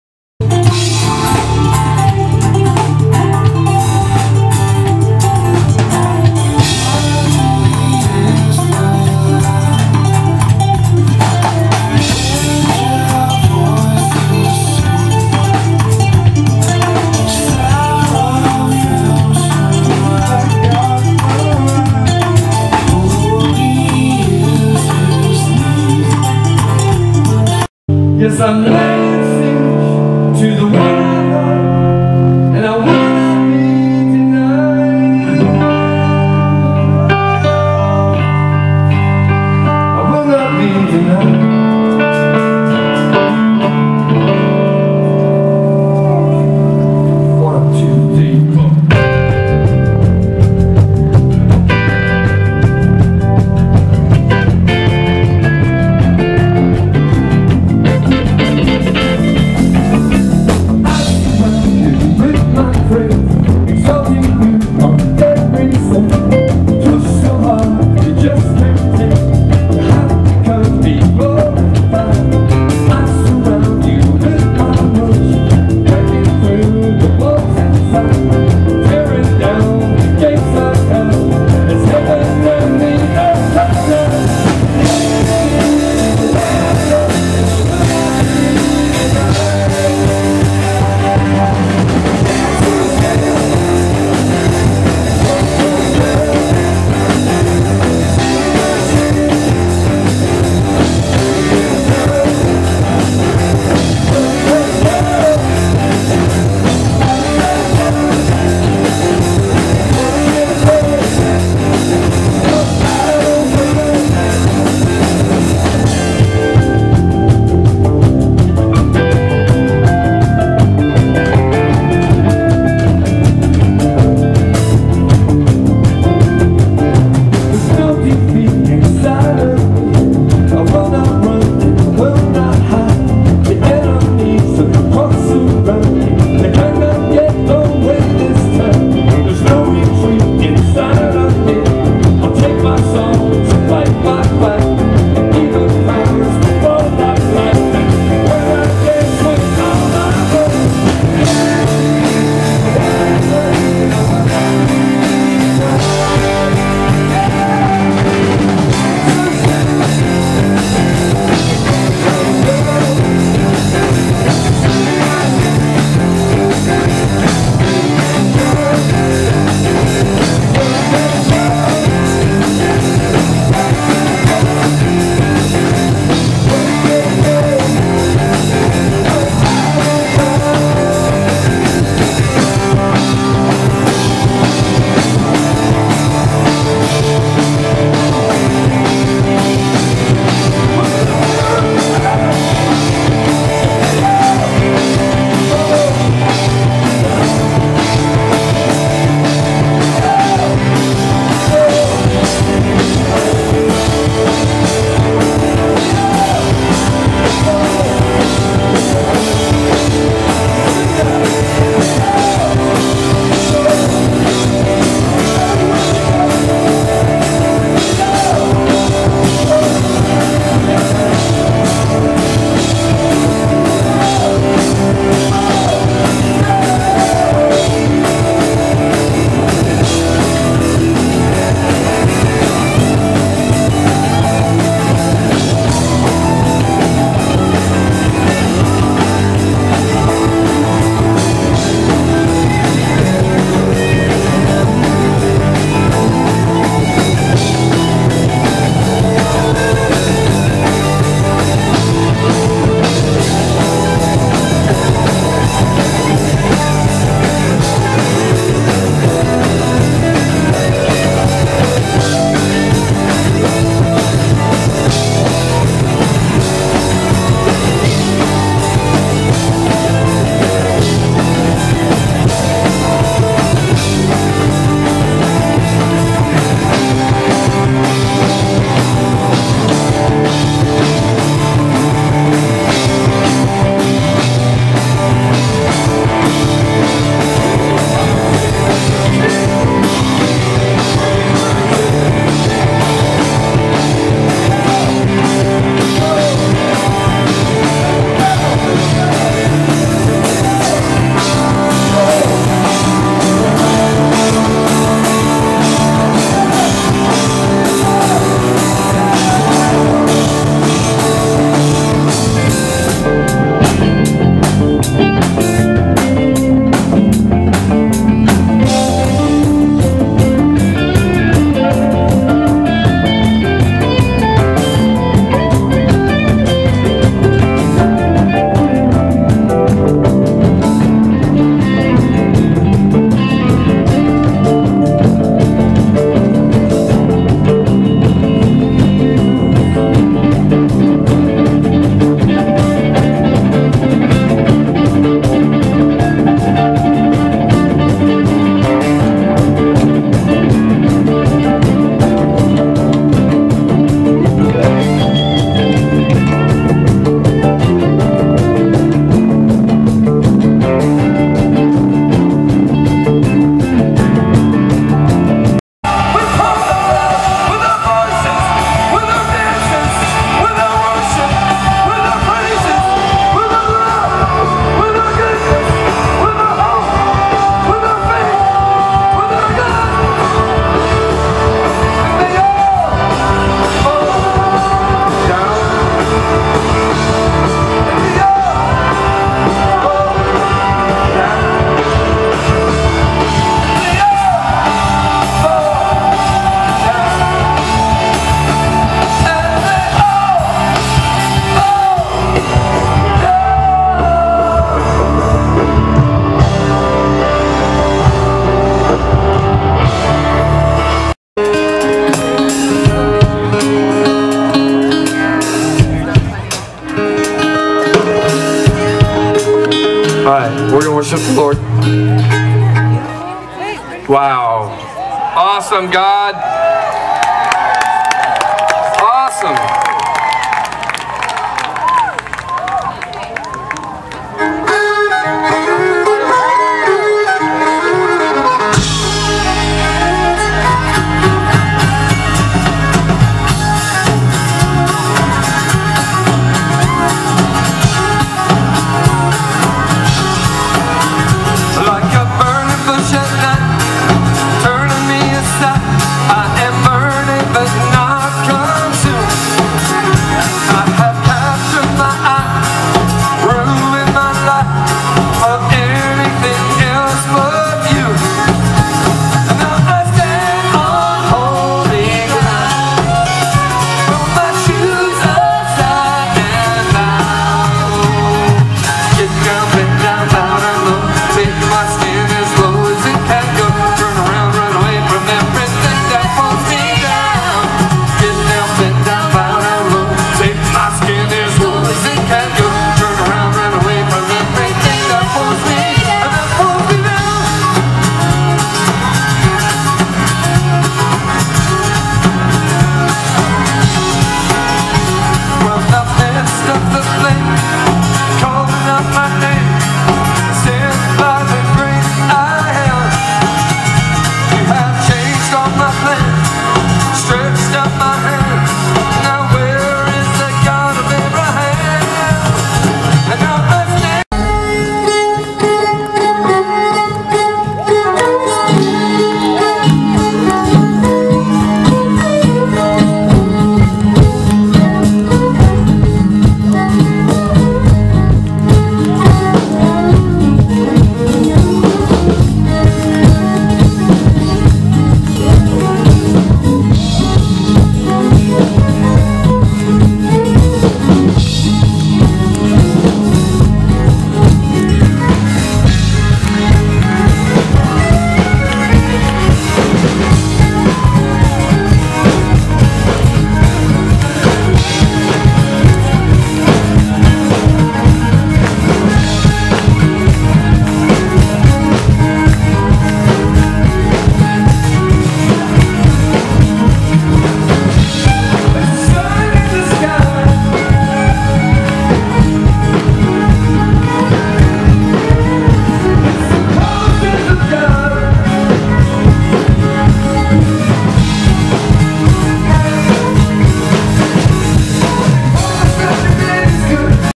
God's Summer of Love- Sedona 07
Cross-country tour, including musicians Leonard Jones,  Luke and Molly Scaggs.  Filmed by Brian Webb, edited by Ron David Metcalf.  @ 200 people at a time participated on various sections of this trek, beginning in Nashville and ending in San Francisco.  Thanks and God bless all who participated and who watch this video.